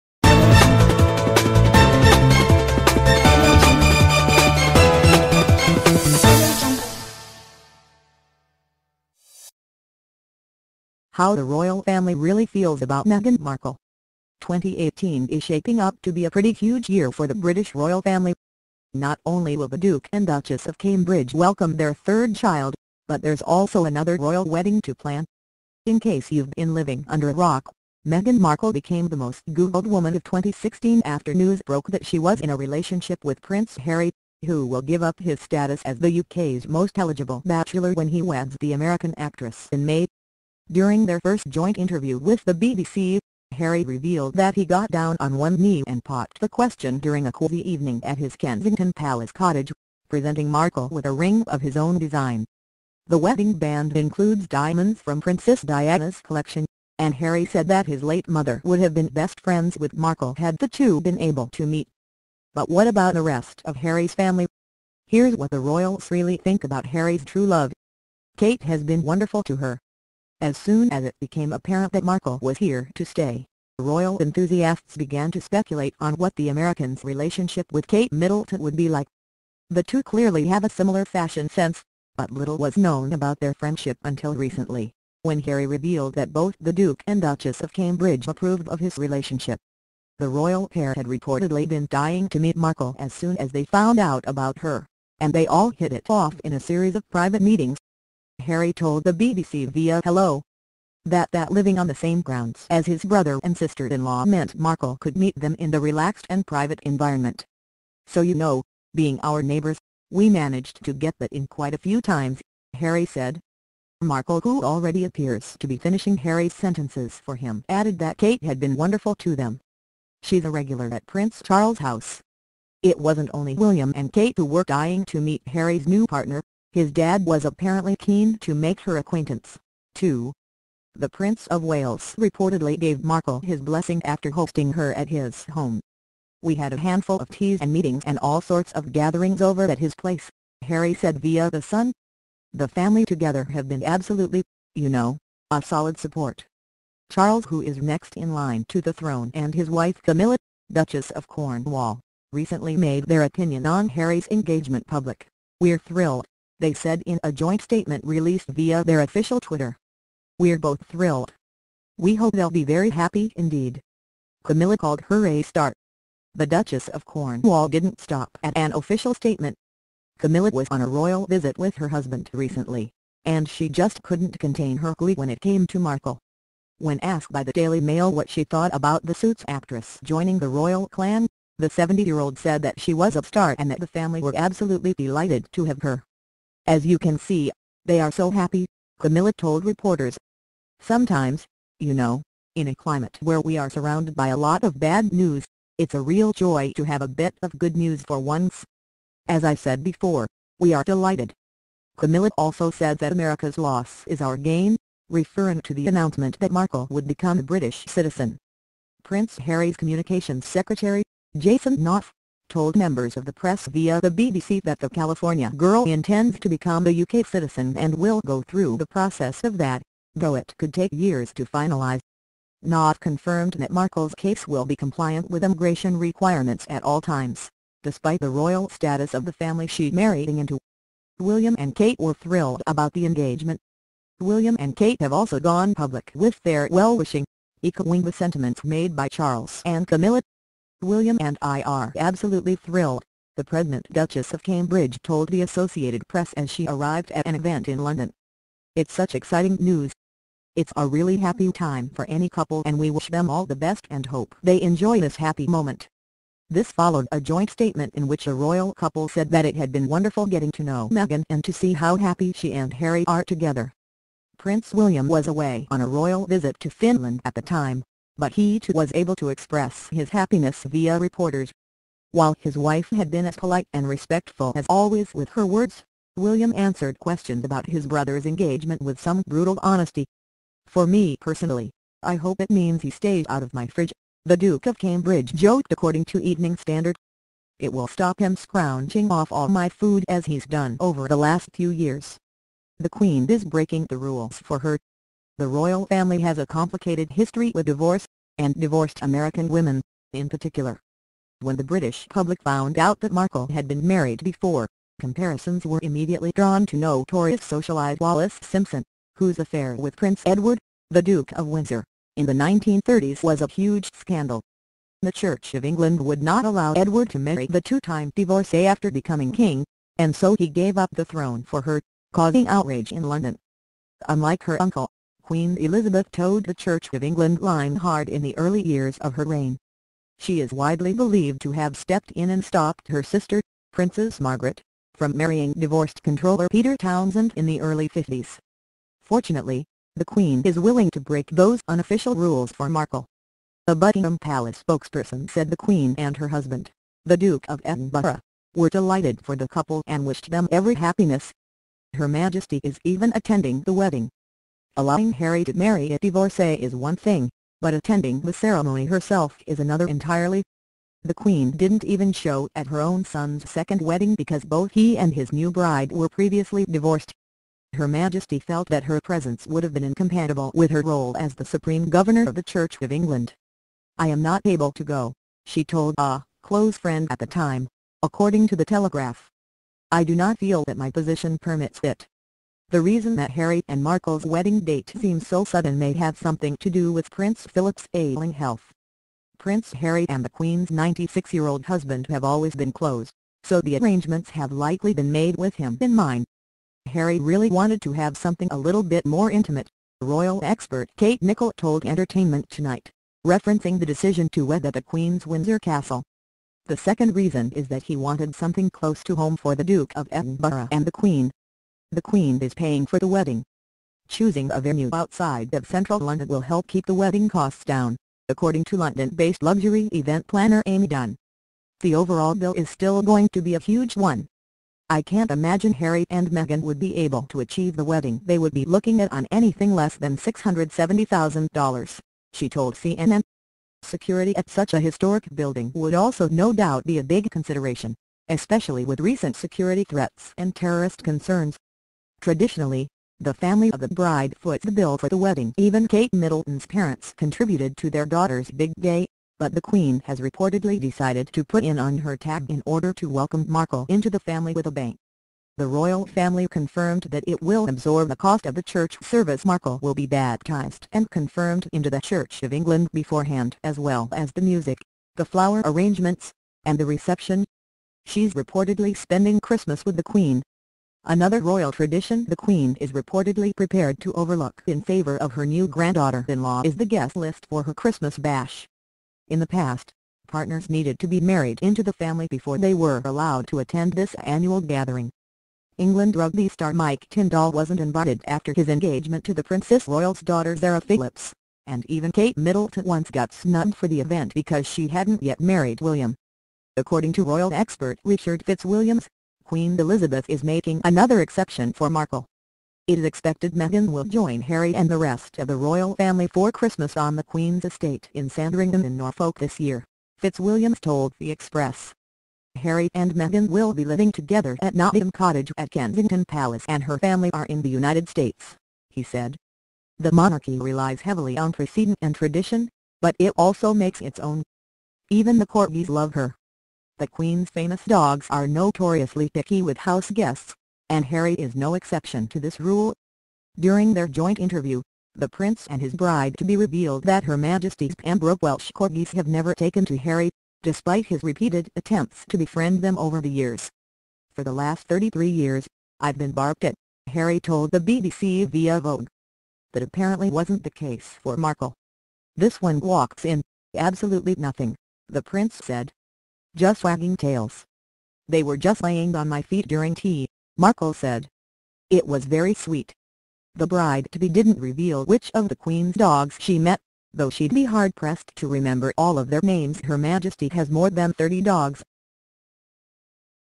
The Royal family really think about Meghan Markle
2018 is shaping up to be a pretty huge year for the British royal family. Not only will the Duke and Duchess of Cambridge welcome their third child, but there's also another royal wedding to plan!